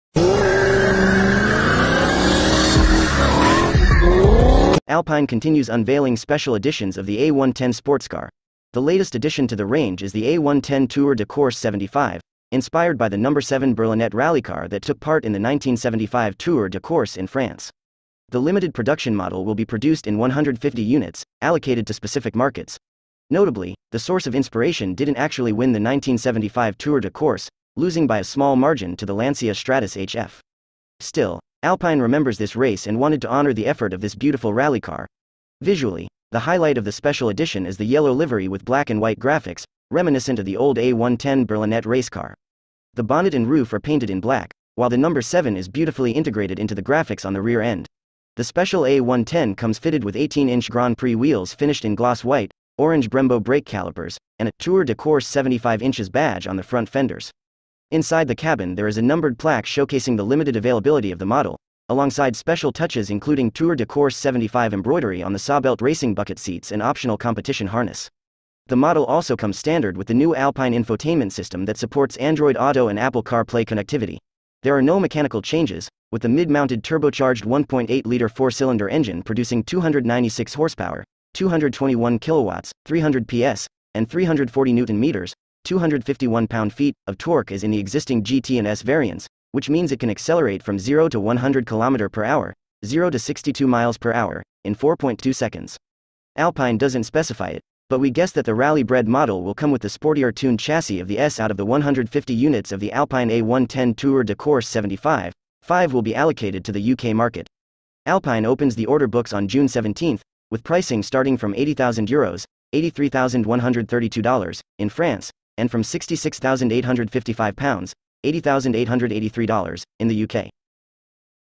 Alpine A110 Tour de Corse
Alpine continues unveiling special editions of the A110 sportscar. The latest addition to the range is the A110 Tour de Corse 75, inspired by the No. 7 Berlinette rallycar that took part in the 1975 Tour de Corse in France. The limited-production model will be produced in 150 units, allocated to specific markets.

Notably, the source of inspiration didn’t actually win the 1975 Tour de Corse, losing by a small margin to the Lancia Stratos HF. Still, Alpine remembers this race and wanted to honor the effort of this beautiful rally car.